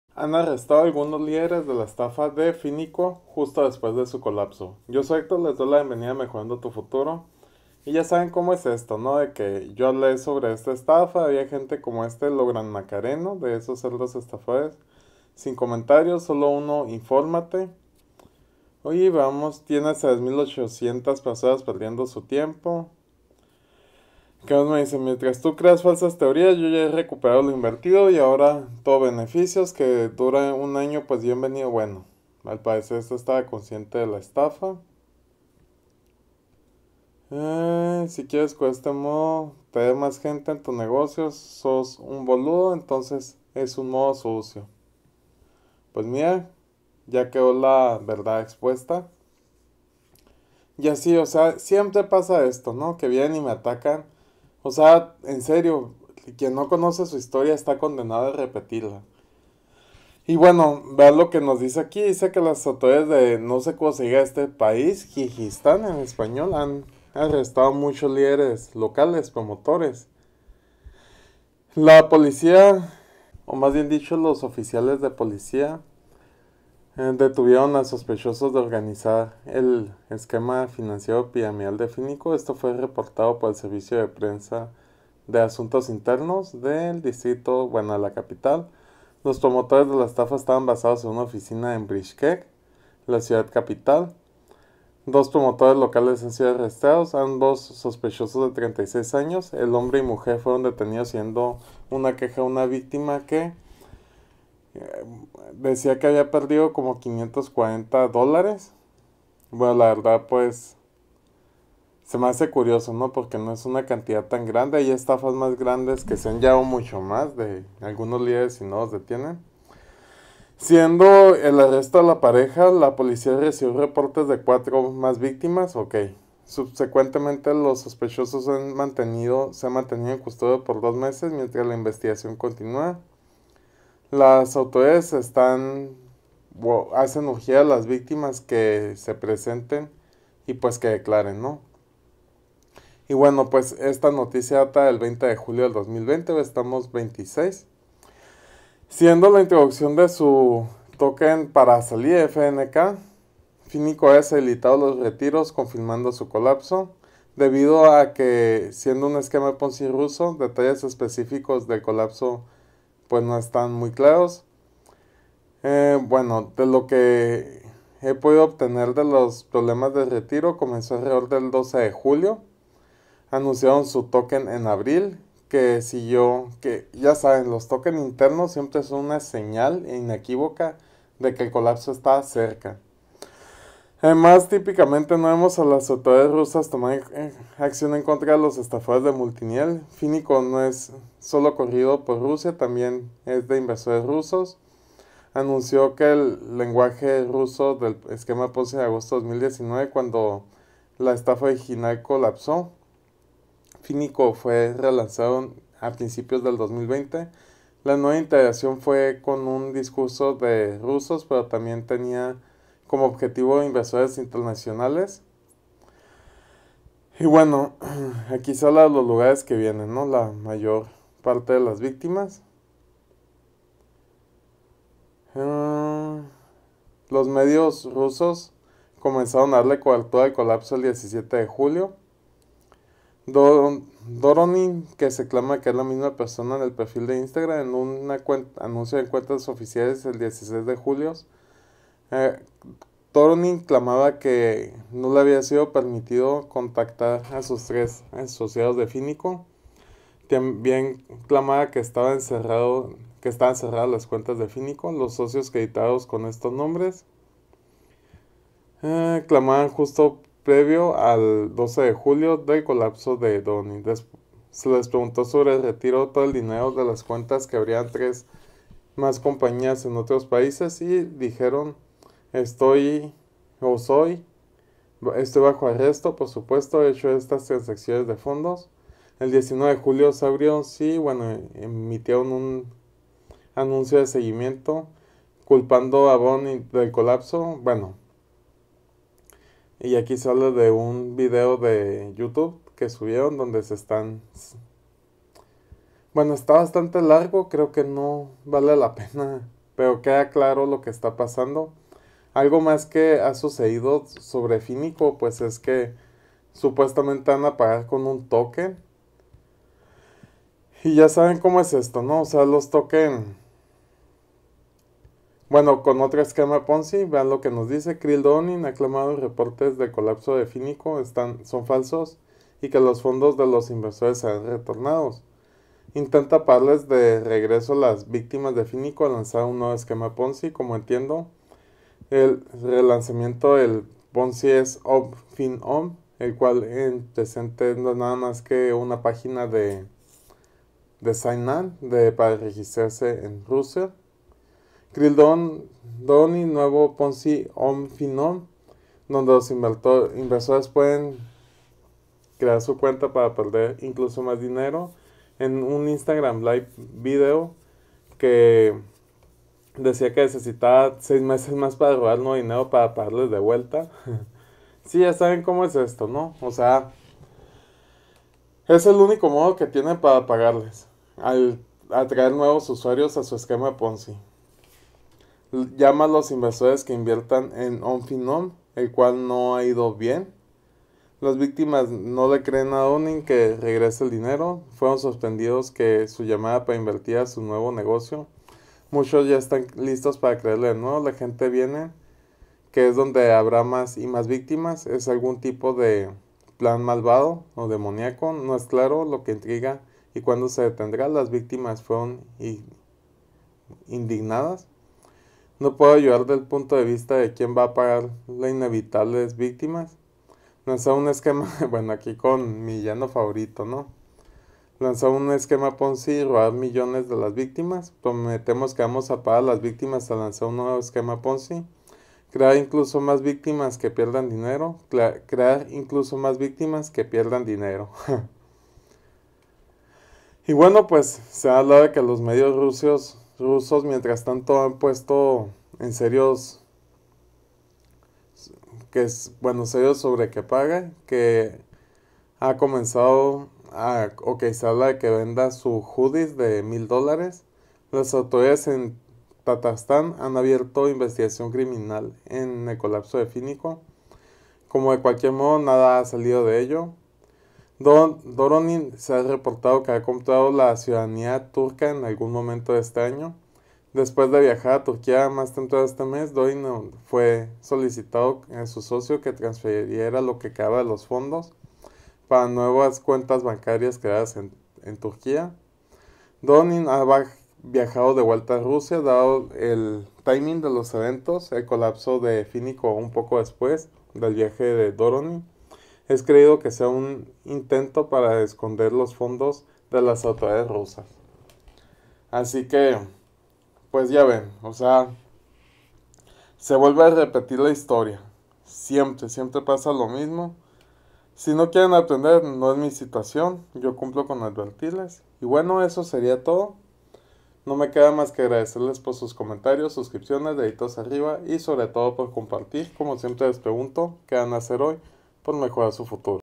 🐀Finiko😈 ha dejado de 💸pagar, detienen a líderes y ¿🤔plan para pagar de vuelta?
La estafa de Finiko ha dejado de pagar, arrestan a líderes y tienen un supuesto plan de repago

Crea tu cuenta gratis en Binance(Plataforma más confiable para trading en criptos)
Contacto en Telegram:  @mejorandotufuturo

¿Más Inversiones?